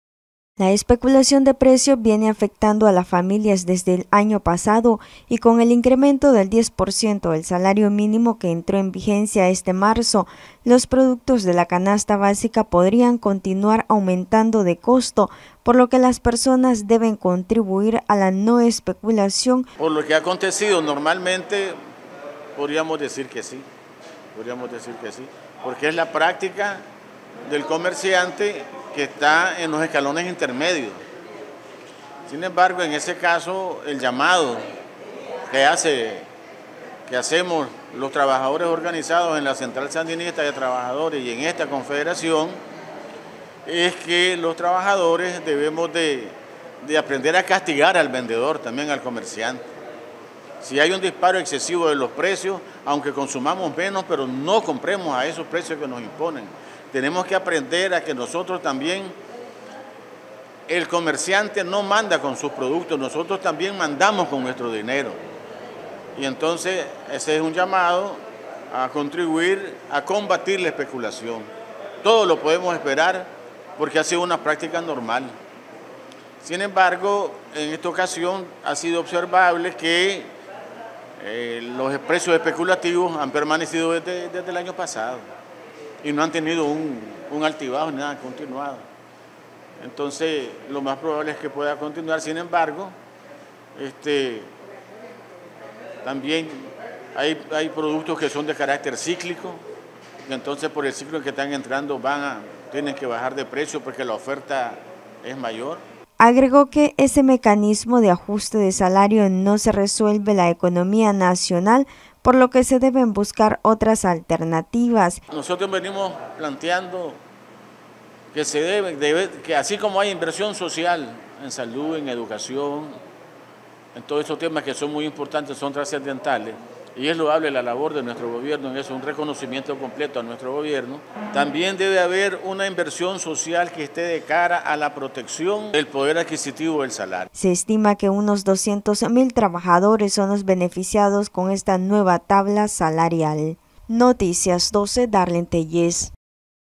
Noticias12Nicaragua Población debe contribuir a la no especulación señaló sindicalista
El secretario de la CST Indalecio Rodríguez que instó a las familias a contribuir a la no especulación, de los productos de la canasta básica que semanalmente aumentan de costo